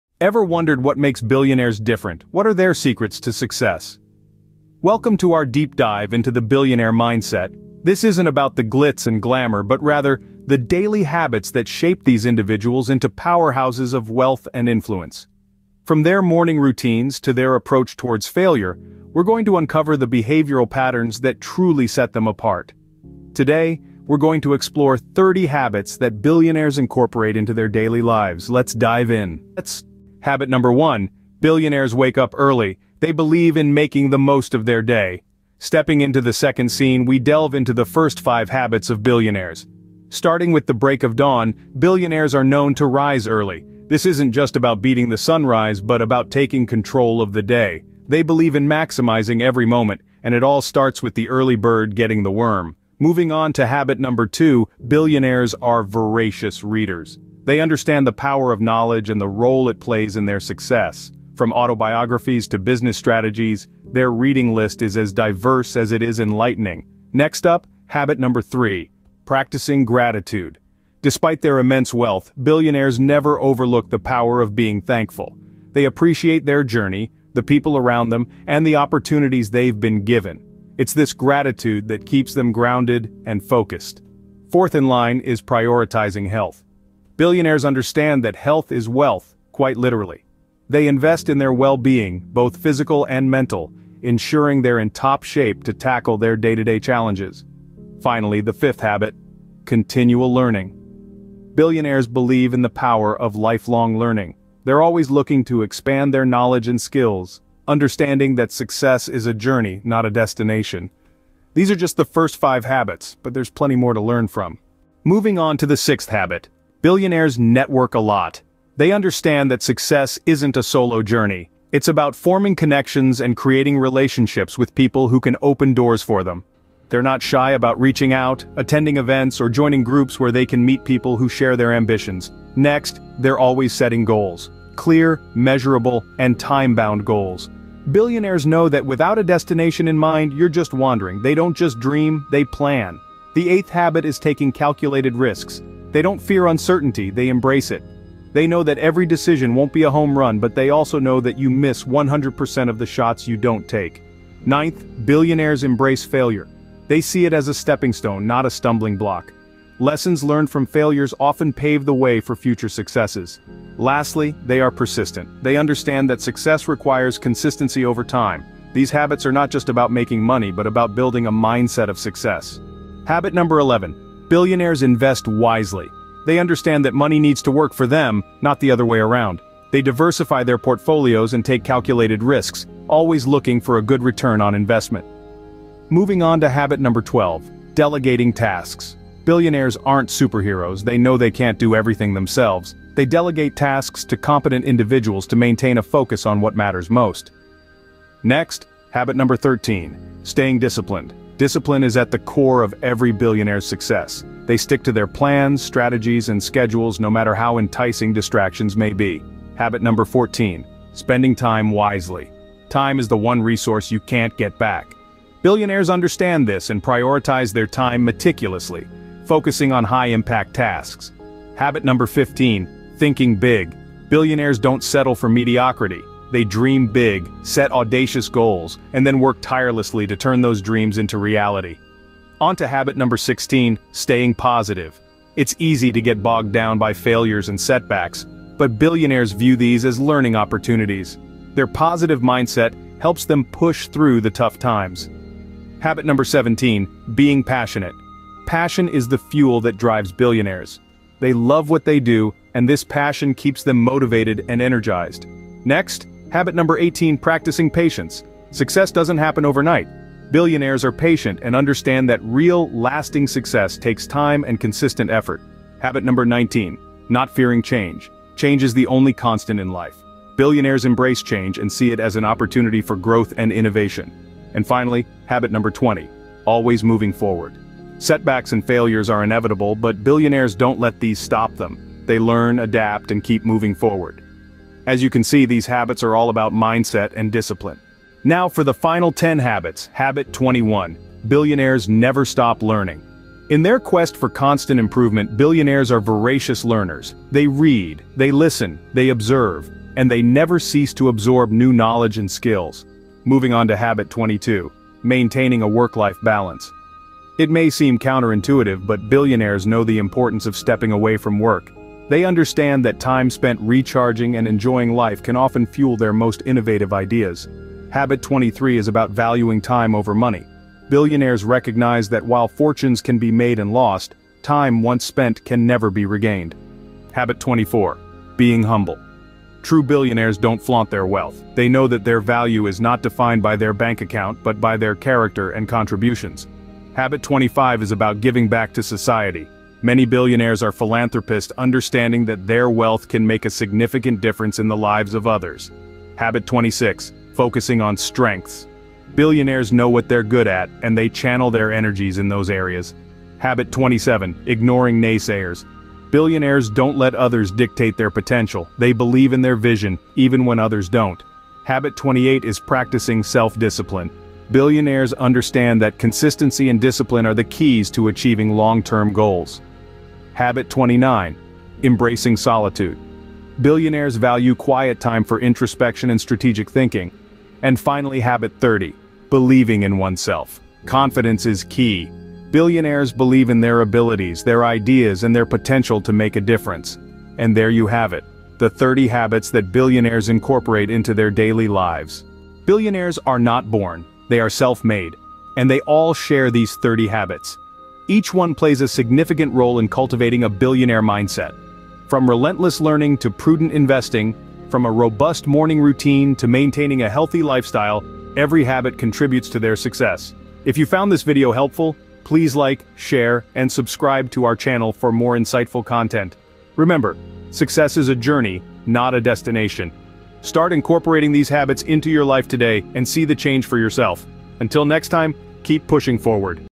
30 Billionaire Habits You Need to Adopt Today
30 Billionaire Habits You Need to Adopt Today

Are you curious about the habits that billionaires possess that have contributed to their success? In this video, we explore the top 30 habits that billionaires incorporate into their daily lives and how you can adapt them to achieve your own goals. From time management and goal setting to mindset and self-care, these habits are key to unlocking your full potential.

Join us as we delve into the habits of the ultra-successful and discover how you can implement them starting today.

OUTLINE:

00:00:00 The Billionaire Mindset
00:00:29 Habits 1-5
00:01:54 Habits 6-10
00:02:52 Habits 11-20
00:04:48 Habits 21-30
00:06:32 Conclusion